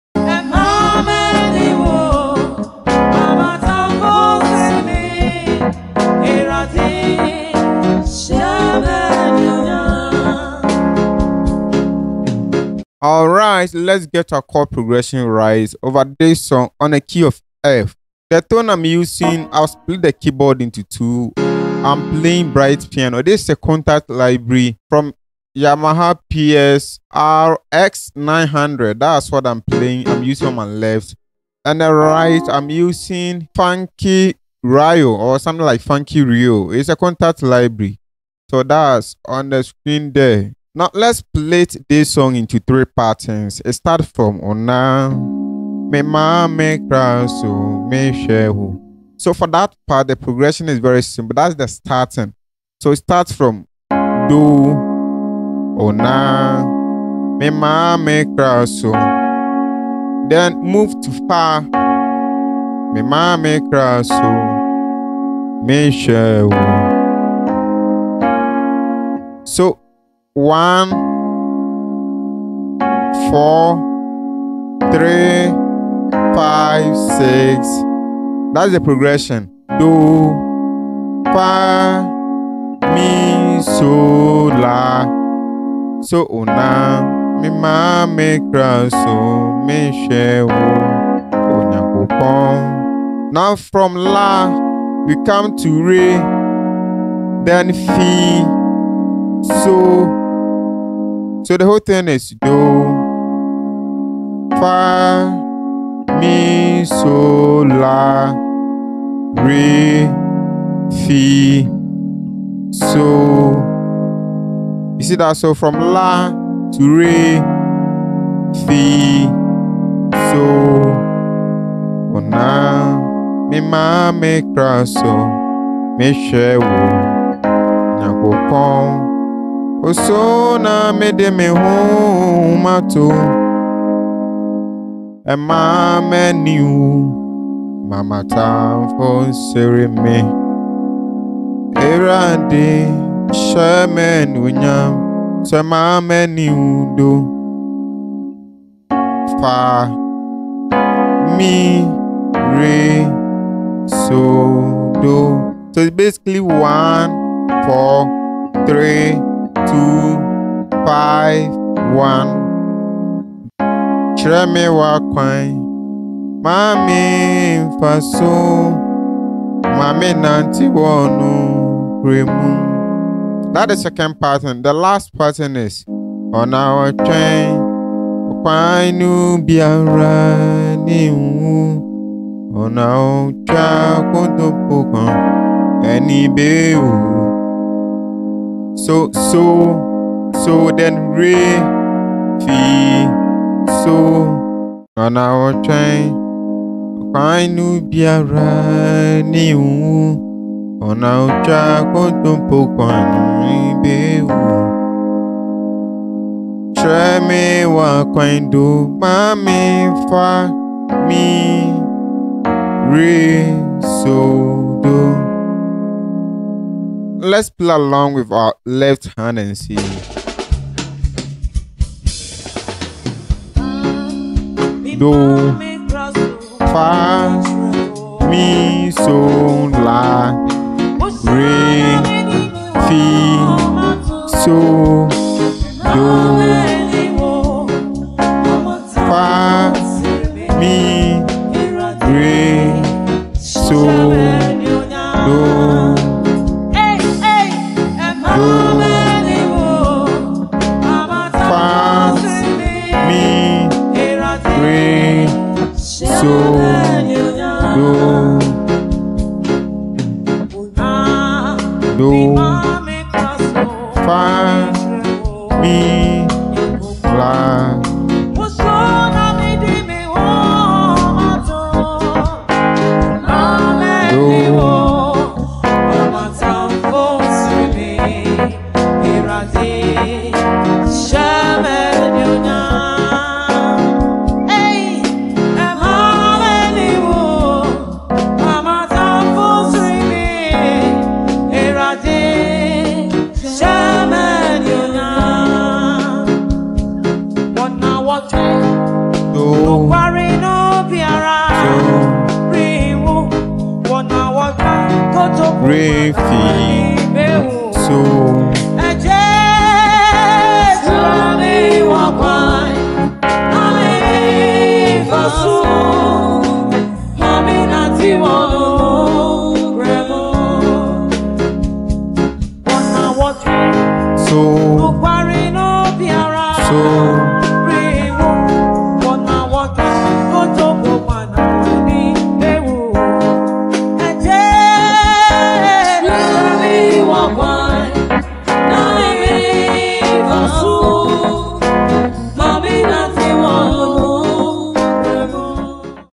Master the Correct Progression of this Song in Key F
In this music theory tutorial, we unravel the mysteries behind the correct chord progression of a song in the key of F. Understanding how to construct a compelling musical journey in this key is crucial for any aspiring musician or songwriter. Join us as we dive deep into the theory, explore different chord sequences, and reveal the secrets to creating a harmonically rich and emotionally captivating arrangement. Whether you're a beginner or an experienced musician, this video will empower you to master the art of songwriting in the key of F. Don't forget to like, subscribe, and hit the notification bell to stay tuned for more insightful music tutorials and songwriting tips!